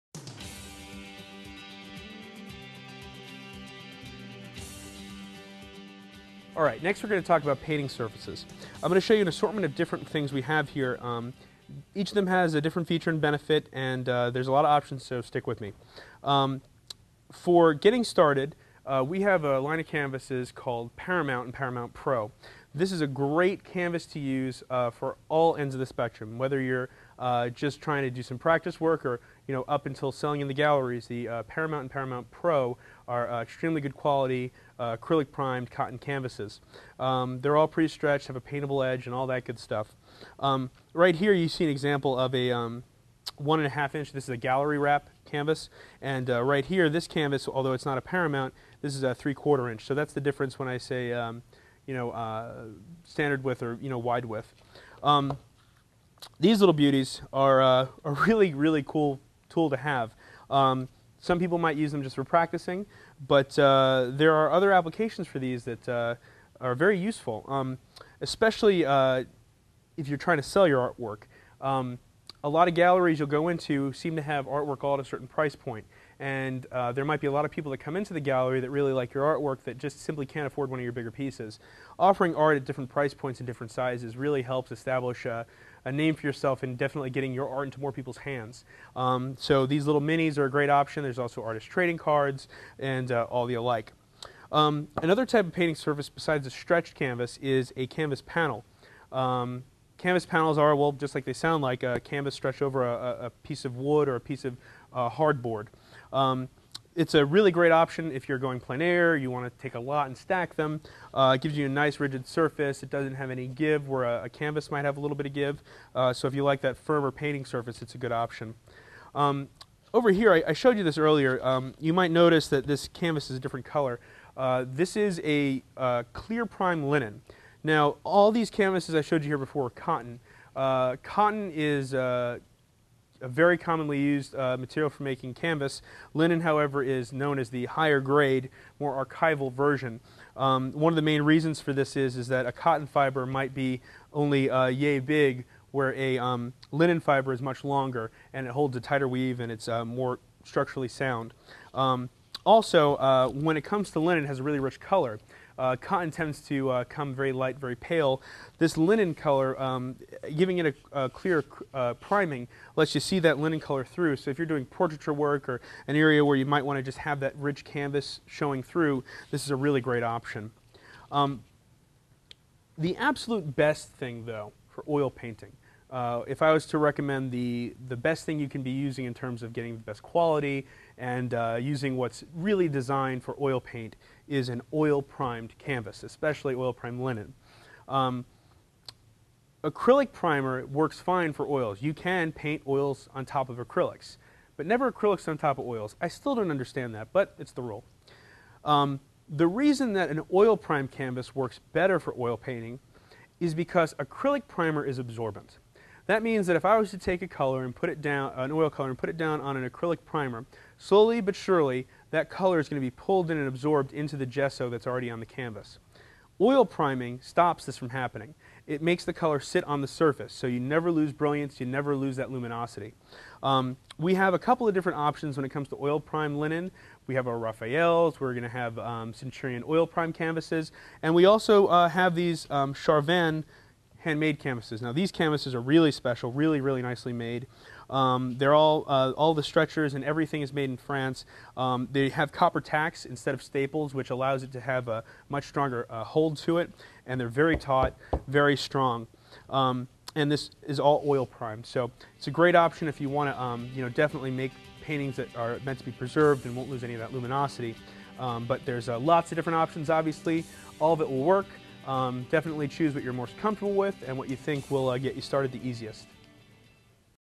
Oil Painter's Studio: Surfaces
Choosing the right painting surface is challenge faced by all oil painters. In this lesson, Mike Goldstein has the insight to help you make the right decision when choosing your canvas or panel.